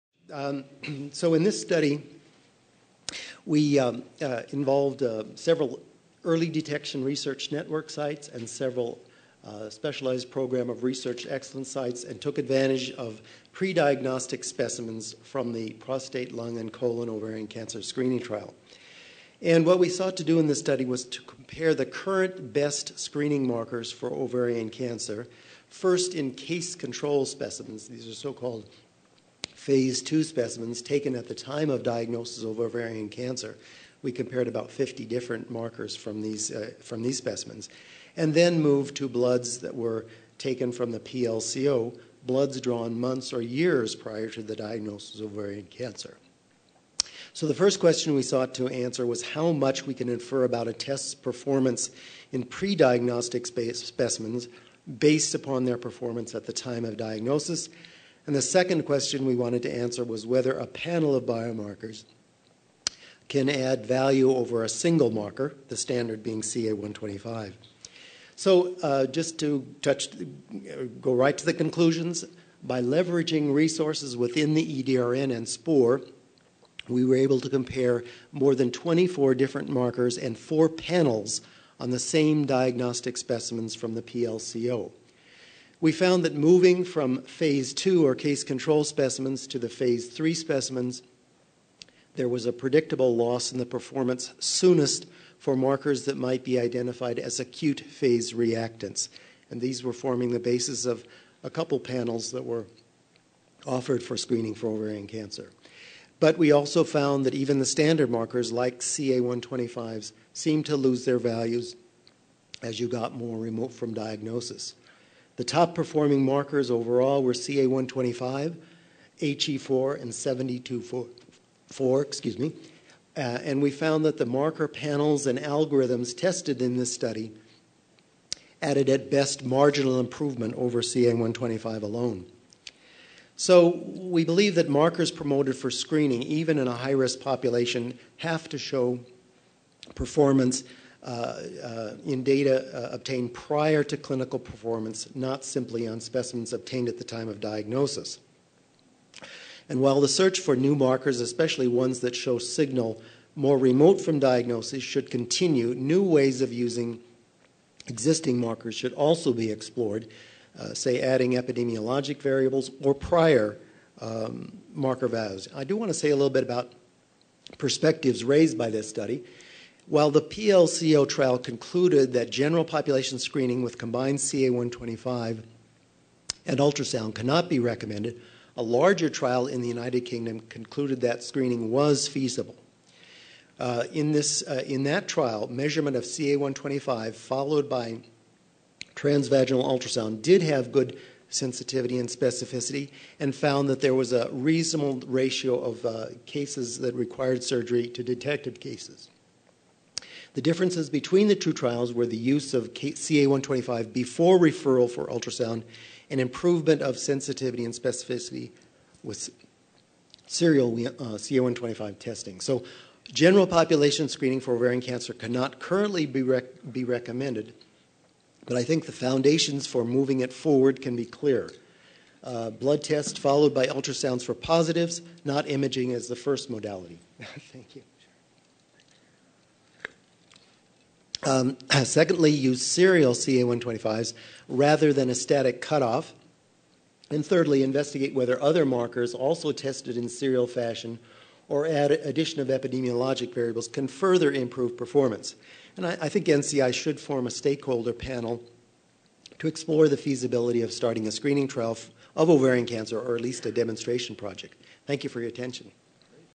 AACR 2009: Ovarian cancer biomarkers
Daniel W. Cramer, M.D., Sc.D., professor of obstetrics and gynecology, Brigham and Womens Hospital, Harvard Medical School, presents at AACR 2009: Phase III Validation of Ovarian Cancer Biomarkers in Pre-Diagnostic Specimens from the PLCO Screening Trial